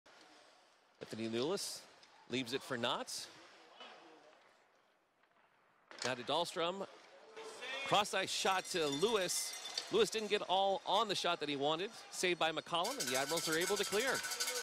McCollum Goes Right to Left to Make the Save
Tom McCollum goes right to left to turnaside a point-blank Rockford chance and keep the game scoreless.